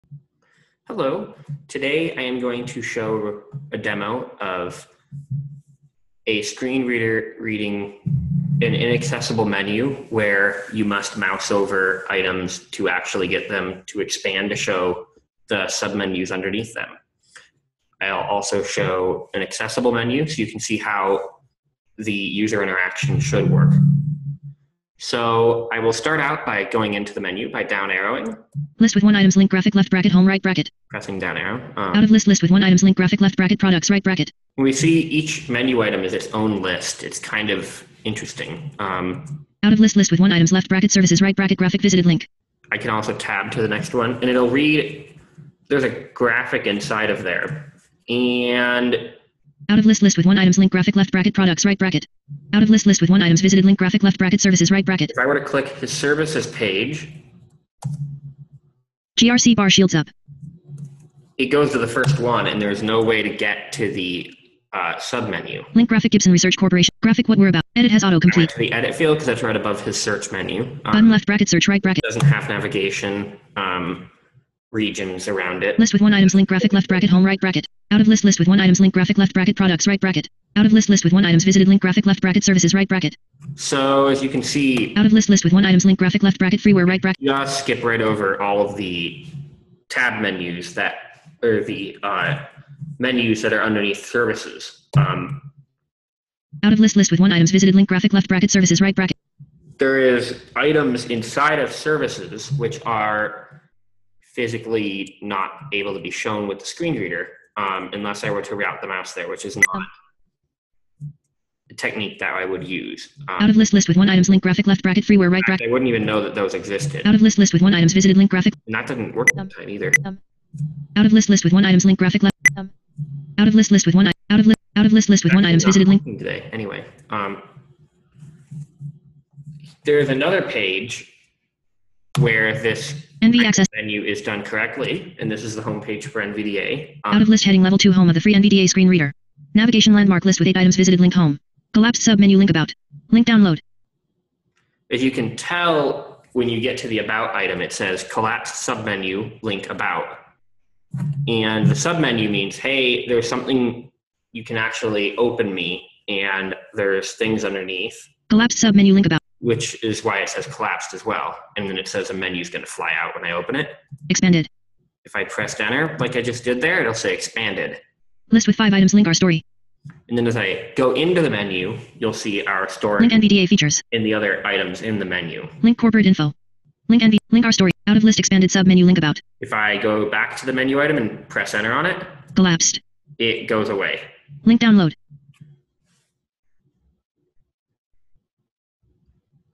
Accessible Drop Down Menus with a Screen Reader
Accessible and inaccessible drop down menus are demonstrated using a screen reader.

To view a transcript of this video:
Click on the More actions button (represented by 3 horizontal dots) located next to the Share button.
Now click on the Transcript option.
A transcript of the closed captions will automatically be generated.